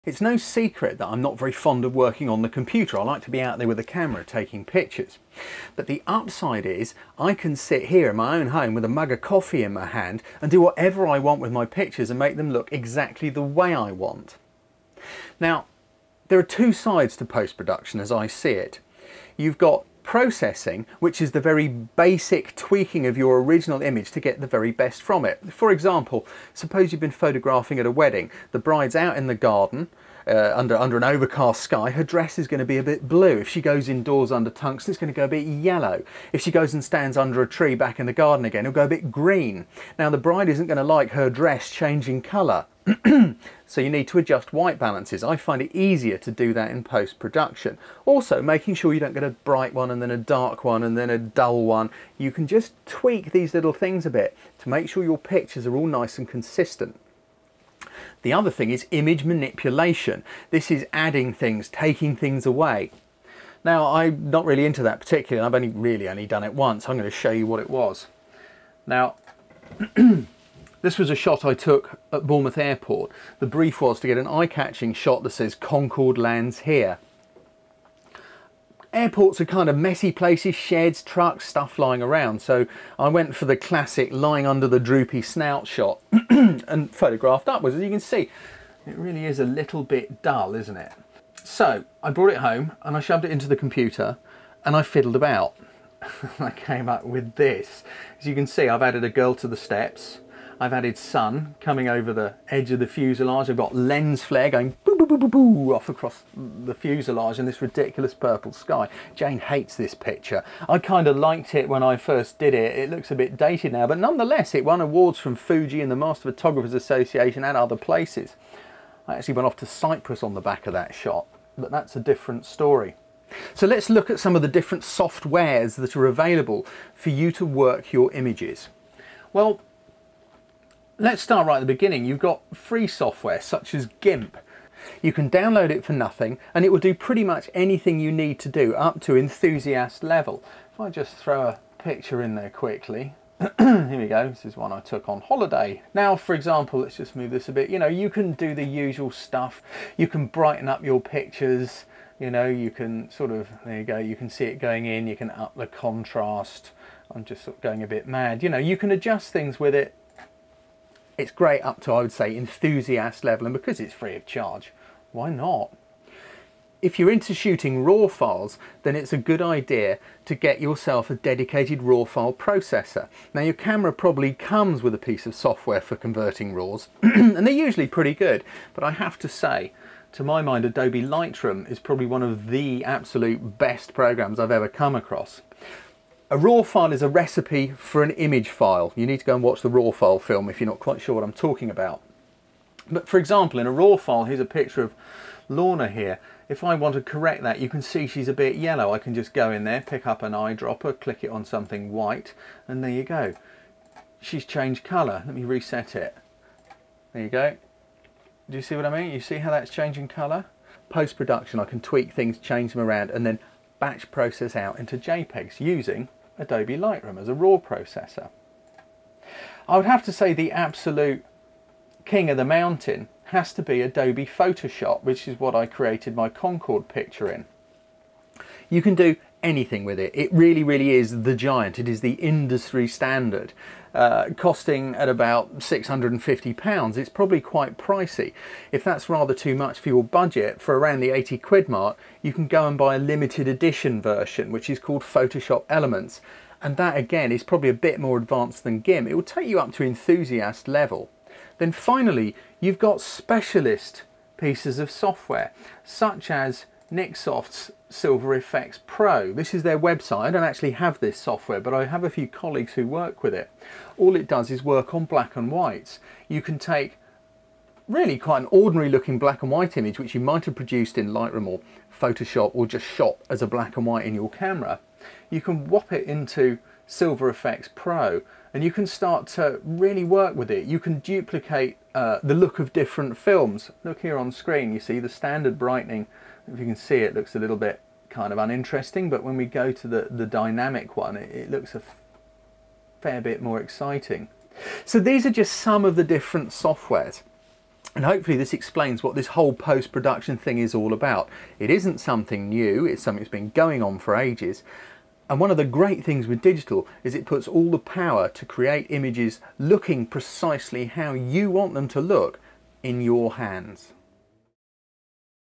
Intro to Photo Editing Pt 2
In Intro to Photo Editing Pt 1 we saw how photo editing has been going on ever since the very beginning. Now lets take a look into the world of digital photo editing. On one hand you have to do the work yourself and if you're not into the computer side of this it's a drag. The upside is that you have complete control over how your images look.

There's a big difference between photo editing and photo manipulation and I'll show you in the video. We'll also look at some of the photo editing software available from free programs like Gimp through Raw processing software and on to Photoshop. There's also specialist software like Silver Effects Pro which can simulate the look of black and white film.

Mike Browne